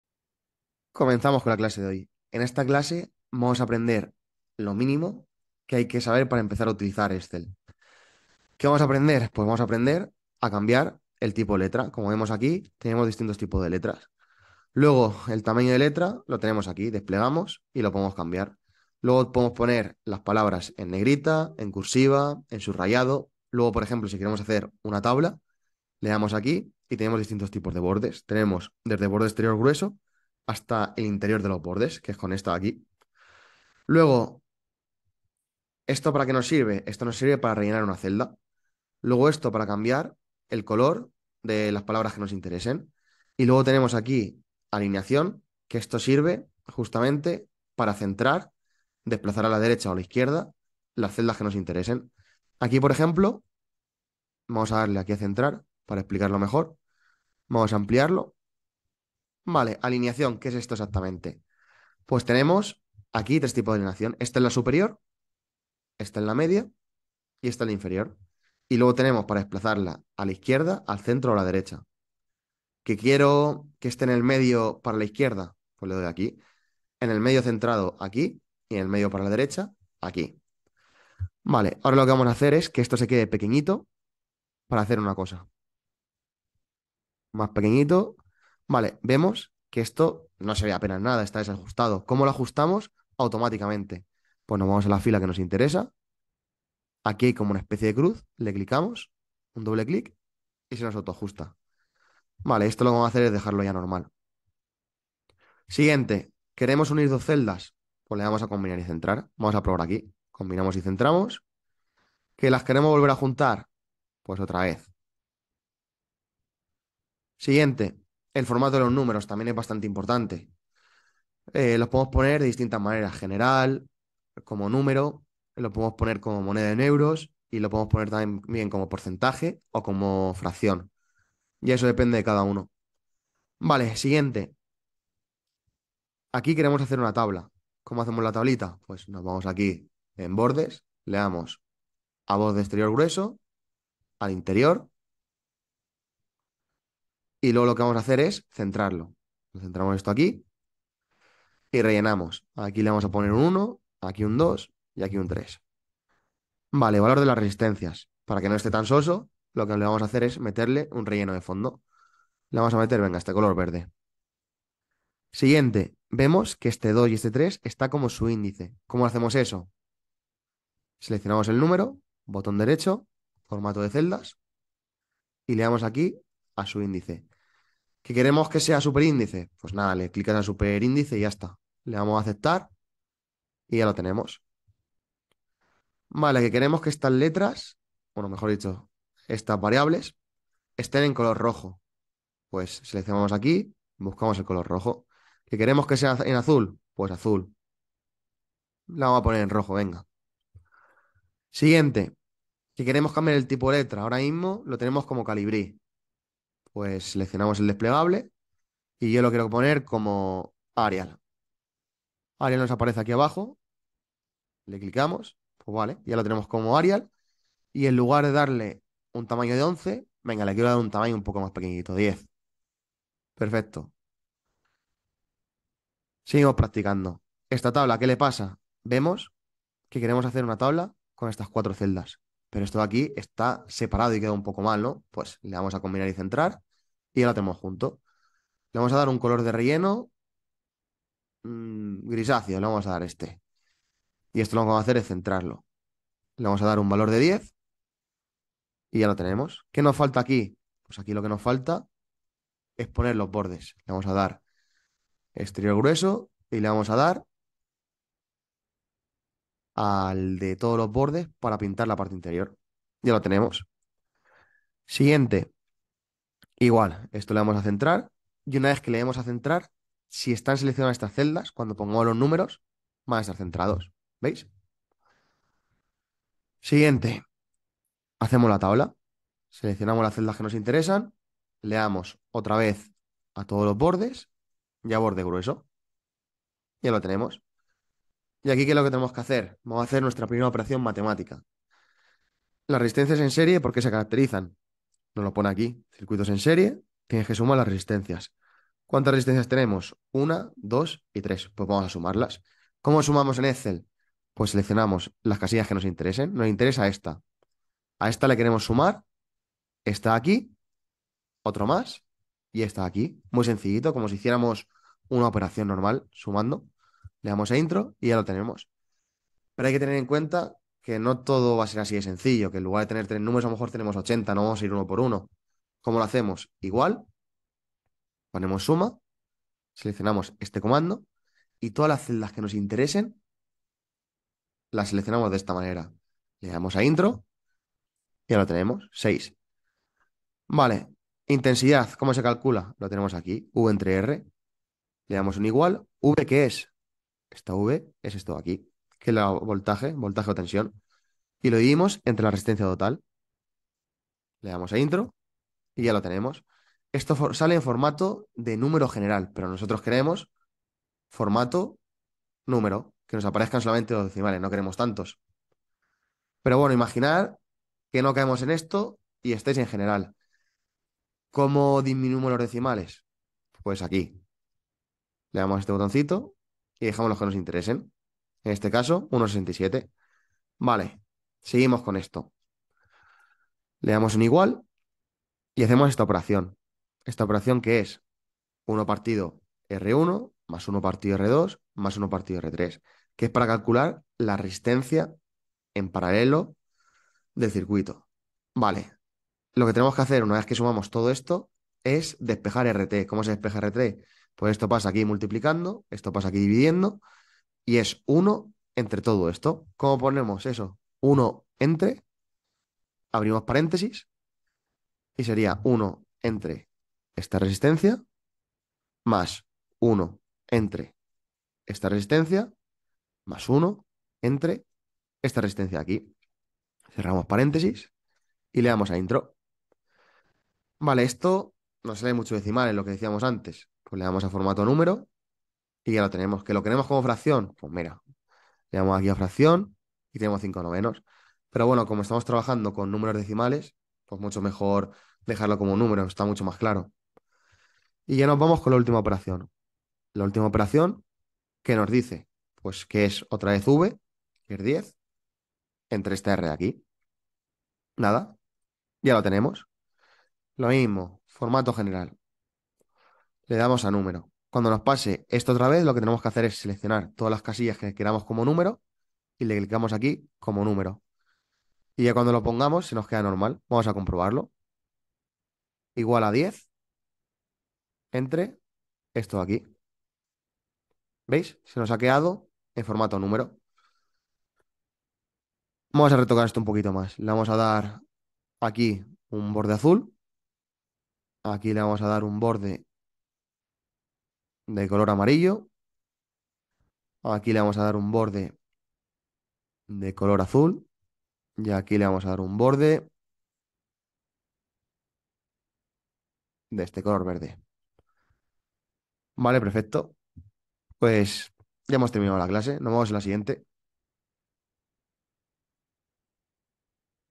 💻💻Aprendiendo por primera vez Excel
Aprende a cambiar el tipo de letra, tamaño, combinar celdas, poner subíndices y superíndices, relleno de celdas, poner bordes, color a las palabras, tus primeras operaciones matemáticas. etc.